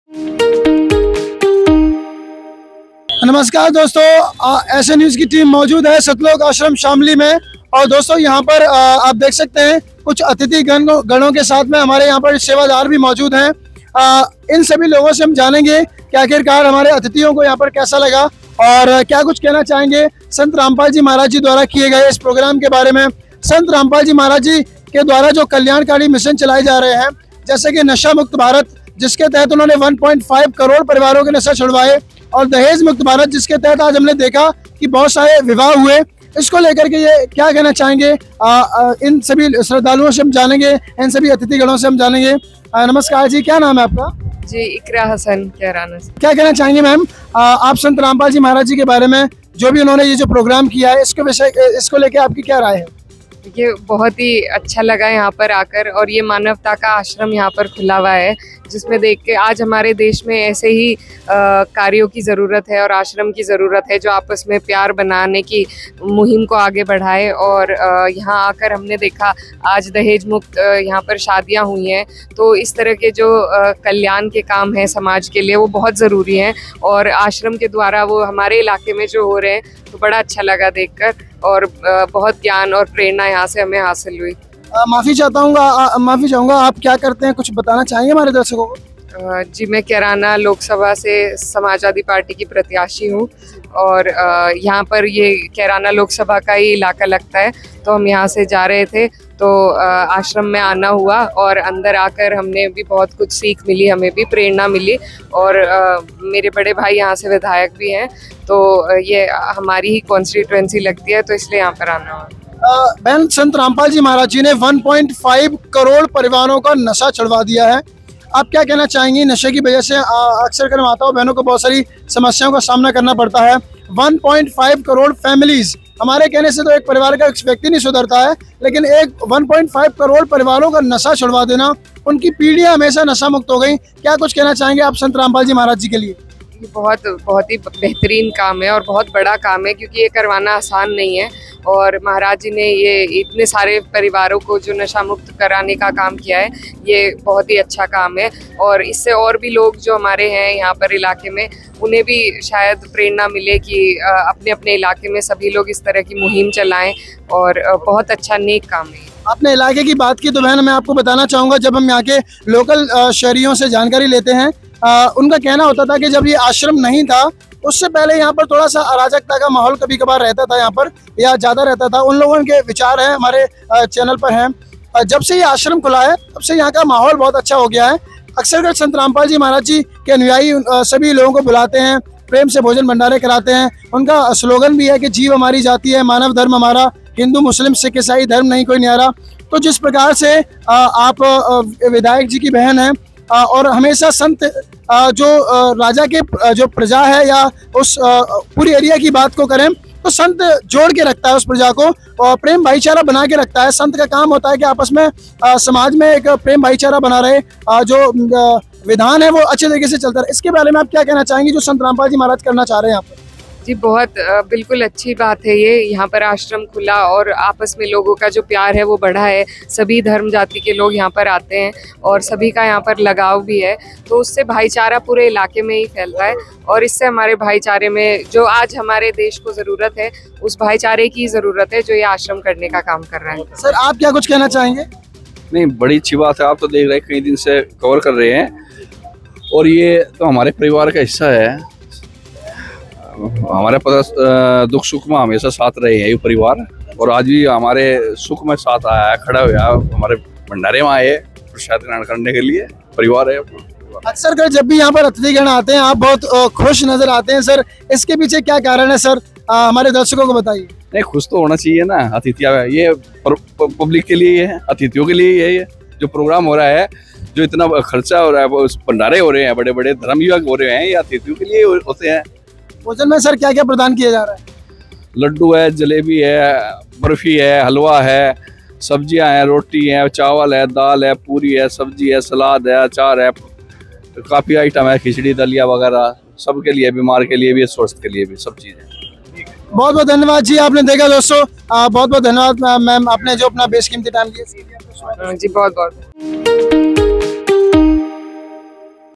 Satlok Ashram Shamli के बारे में क्या विचार थे सपा की युवा महिला नेता Iqra Hasan के | SA NEWS
Join the SA News team as we visit Satlok Ashram in Shamli. In this video, we interact with guests and volunteers to gather their thoughts on the programs organized by Sant Rampal Ji Maharaj. Learn about the impactful initiatives such as the drug-free India mission, which has helped 1.5 crore families overcome addiction, and the dowry-free marriages that took place. Hear testimonials from visitors, including a candidate from the Samajwadi Party for the Kairana Lok Sabha constituency, who shares her insights on the positive changes brought about by the ashram. Witness the unity and love promoted by the ashram and the inspiring work being done to foster brotherhood in the community. Don't miss this heartwarming coverage of social welfare and community service in action.